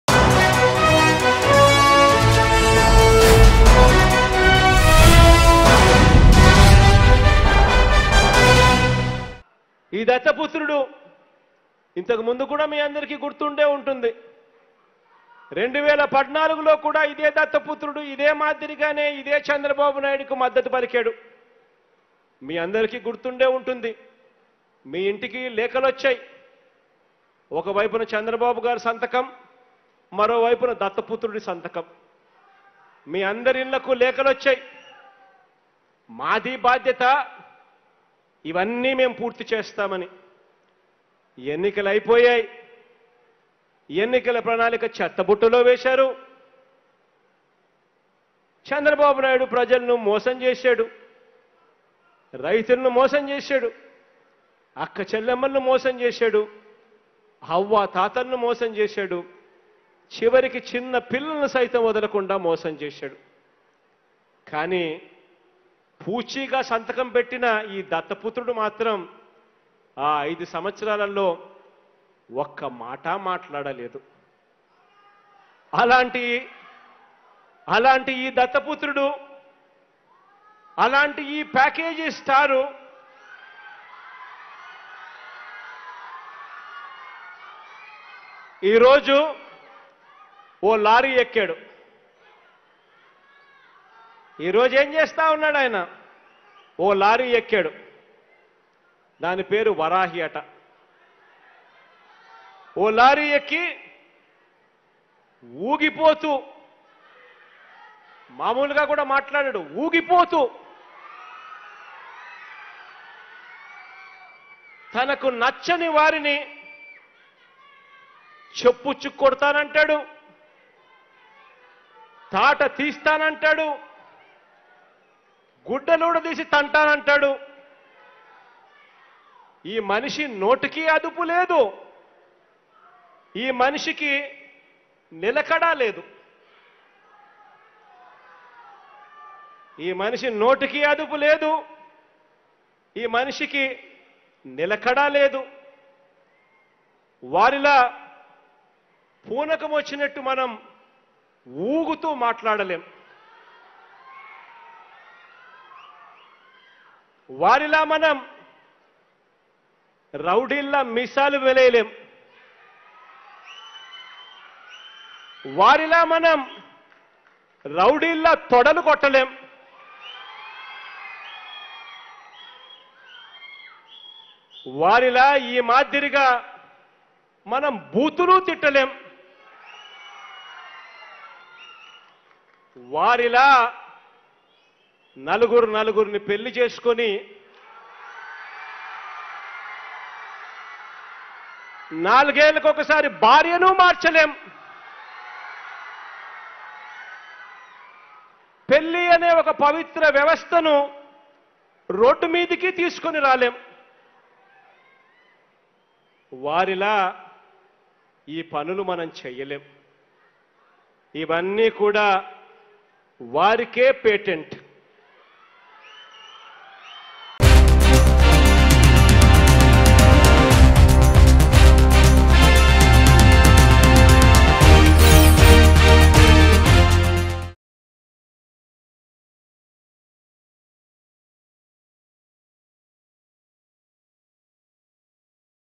CM Jagan Satirical Comments On Pawan Kalyan Varahi Yatra | YSRCP vs Janasena | AP News | Mango News
CM Jagan Satirical Comments On Pawan Kalyan Varahi Yatra.